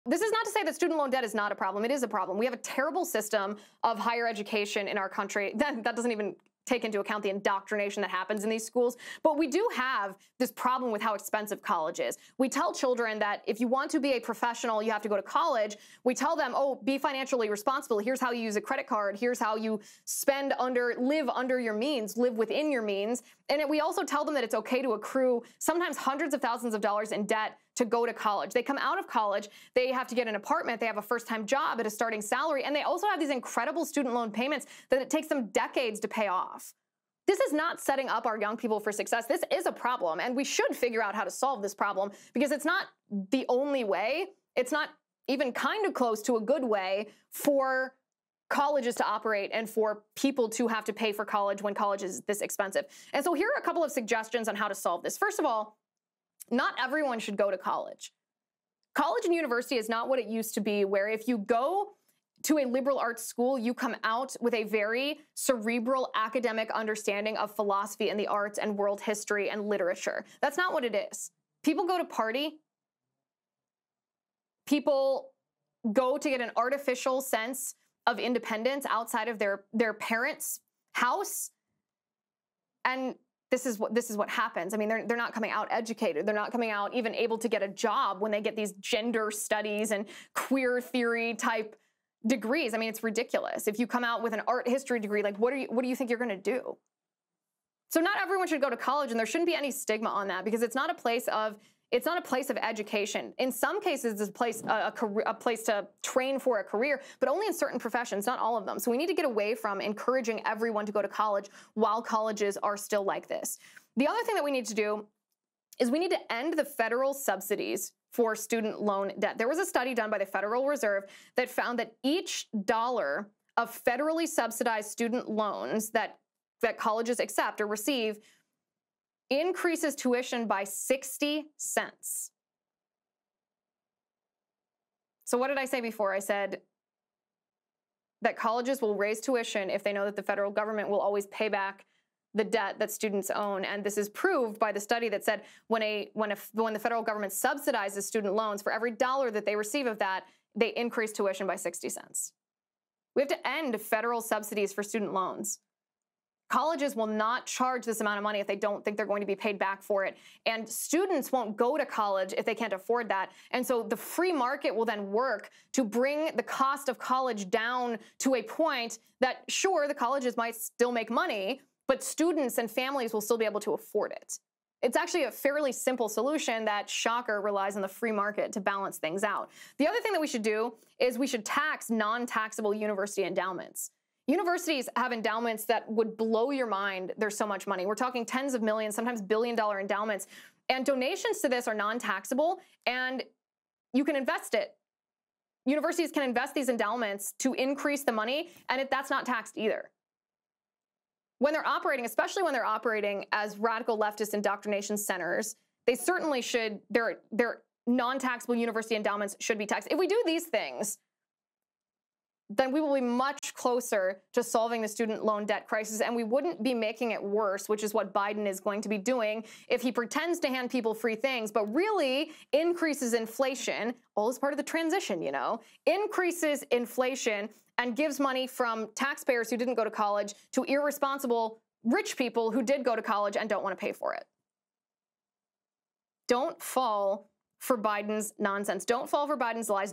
3 ways to fix America’s student loan debt problem
We have a terrible system of higher education in our country that is not setting up young people for success. Here are a few suggestions on how to actually solve this problem.